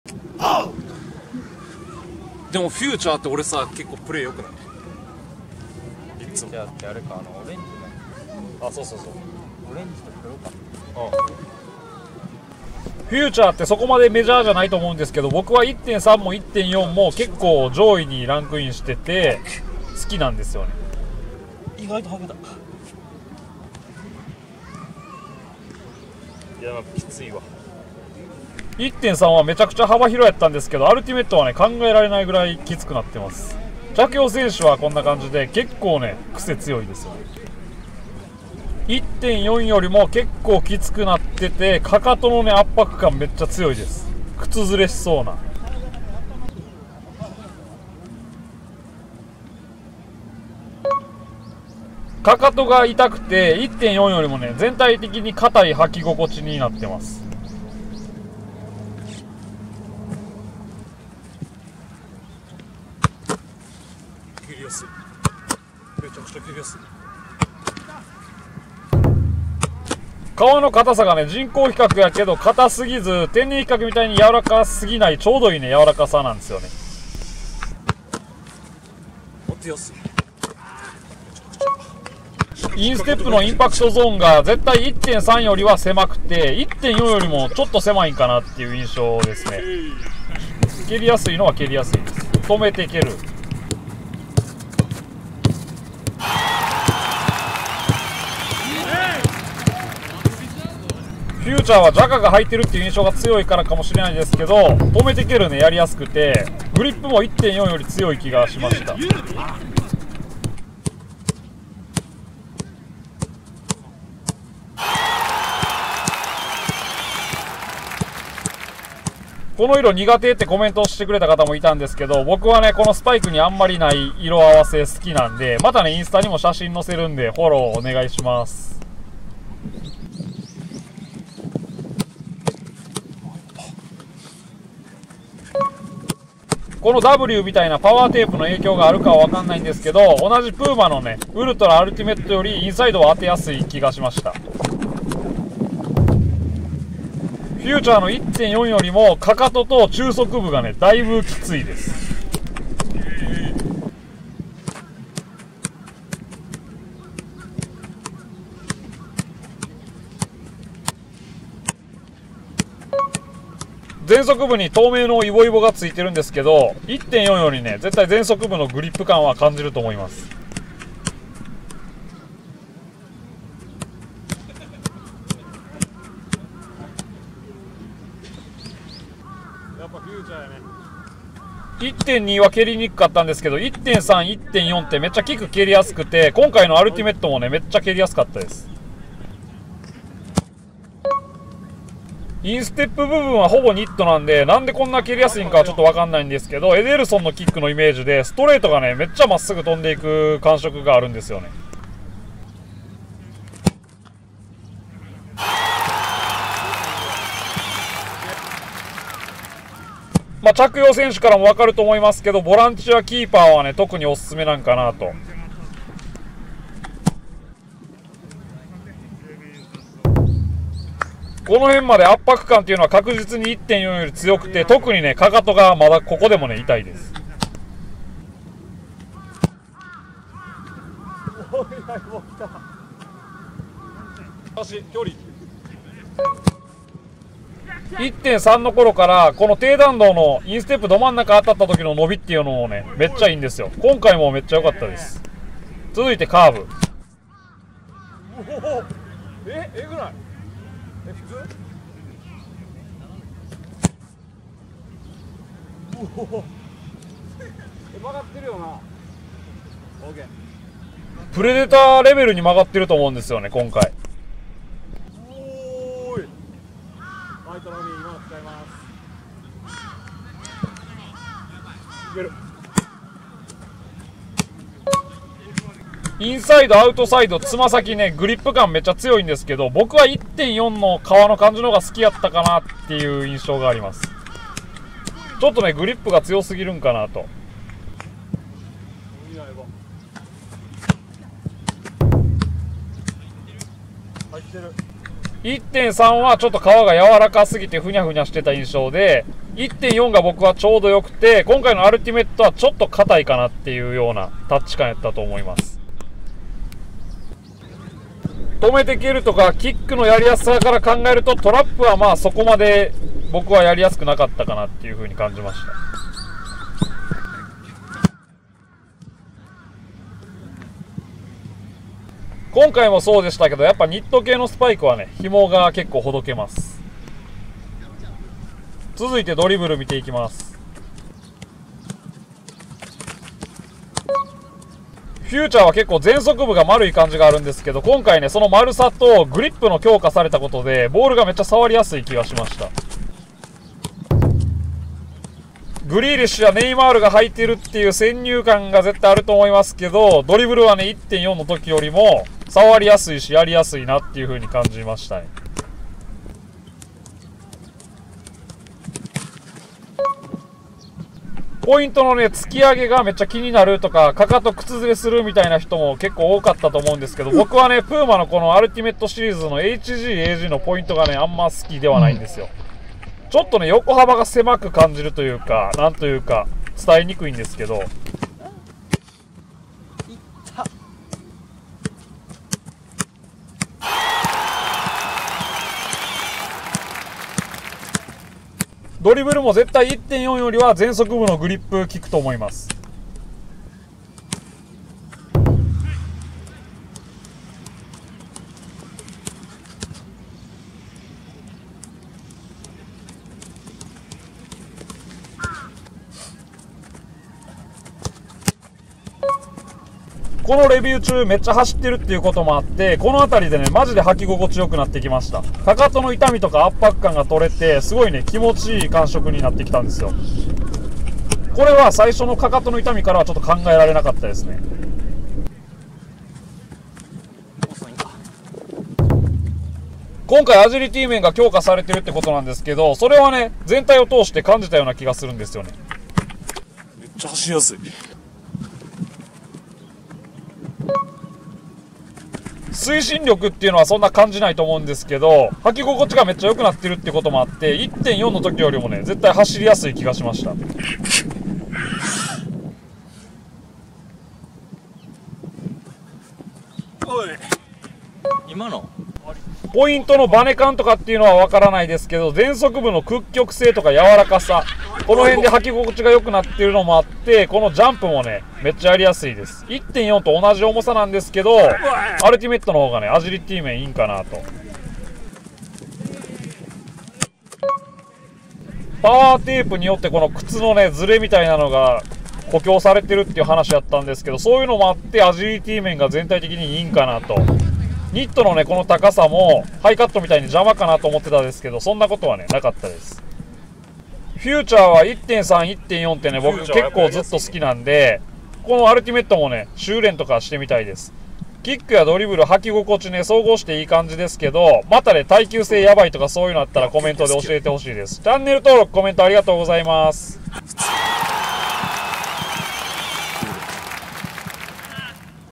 サッカースパイクプーマフューチャーアルティメットのレビュー！ネイマール、グリーリッシュ着用スパイク！
プーマフューチャーアルティメットhg/agモデルのレビューです！
ドリブル、タッチ、トラップ、アジリティ面が前作よりいい気がします✊

⇒「ゴールドジムの商品や施設を超詳しく紹介してるブログ」⇒